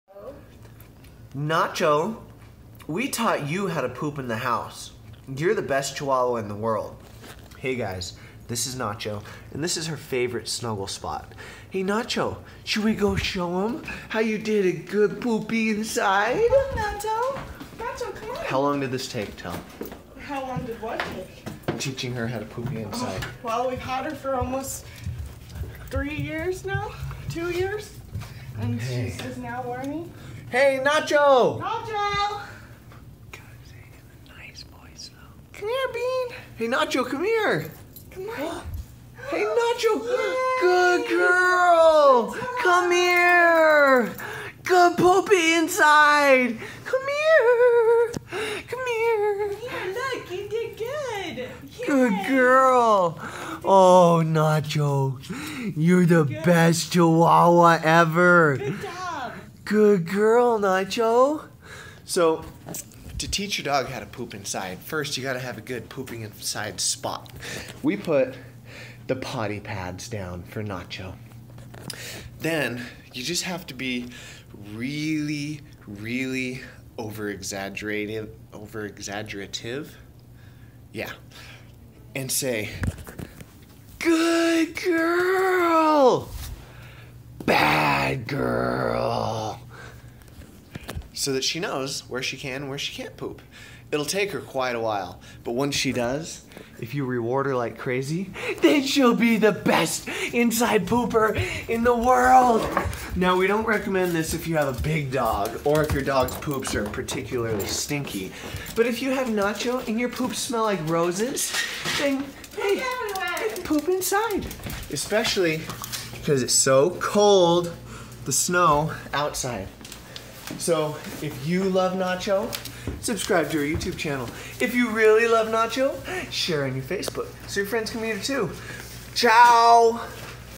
💩 teach your dog to poop inside the house
Nacho cam. YouTube's favorite Chihuahua is back at it again pooping inside the house. The thing is with our dog, her poop smells really good instead of smelling like really bad dog crap.

We attribute this partially to the healthy diet we give our Chihuahua. Every meal has to have high-quality ingredients.

Teaching your dog to poop outside is one trick that pretty much every dog learns. The one thing you learn with Chihuahuas is that they hate the snow and if you live in a snowy winter replace like we do then they hate going outside. So, we just taught Nacho poopy and poop inside on her potty pads because it is so small.